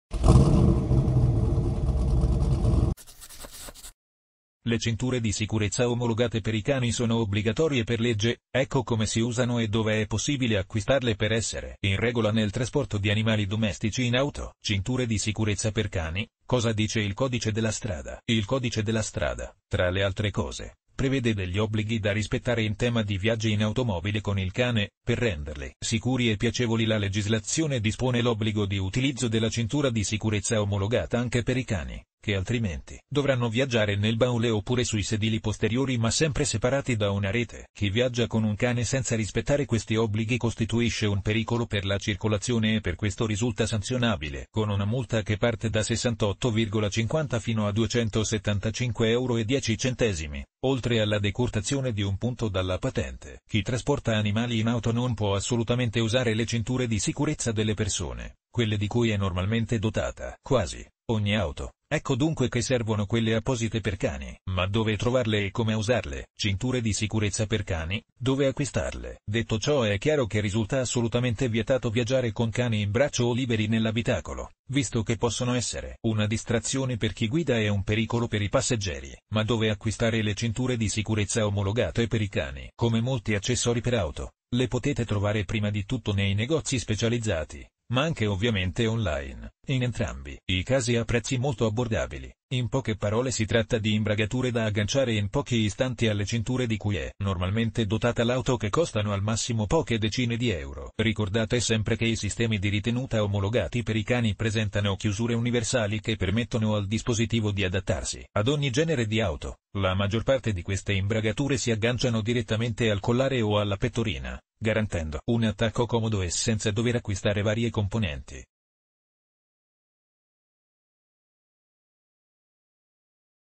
Guida legale: cintura di sicurezza per cani nel codice della strada
Sei un amante dei cani e ti piace portare il tuo amico a quattro zampe in auto? Ma sai cosa dice esattamente il Codice della Strada riguardo all'uso della cintura di sicurezza per cani? In questo video, ti forniremo una guida completa sulle norme legali che riguardano il trasporto dei cani in auto. Scoprirai quali sono gli obblighi del conducente e le regole da seguire per garantire la sicurezza del tuo cane e degli altri passeggeri. Non rischiare multe o sanzioni! Guarda il nostro video e scopri tutto quello che devi sapere sulla cintura di sicurezza per cani secondo il Codice della Strada.